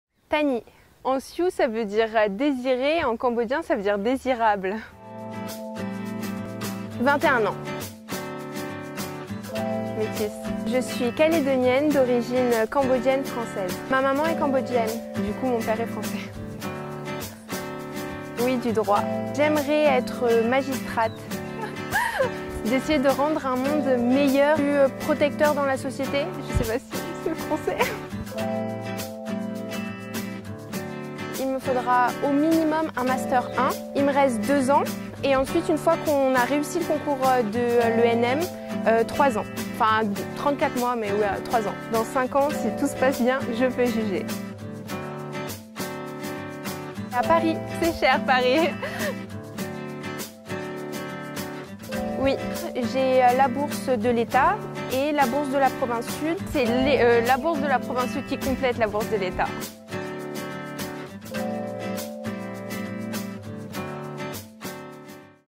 Bourse scolaire de la Province Sud - Tanhy
La Province Sud a lancé sa campagne d’aides aux études supérieures! Tahny vous explique un petit peu son parcours et ses ambitions  😁

N'attendez plus pour faire les démarches, en plus tout est en ligne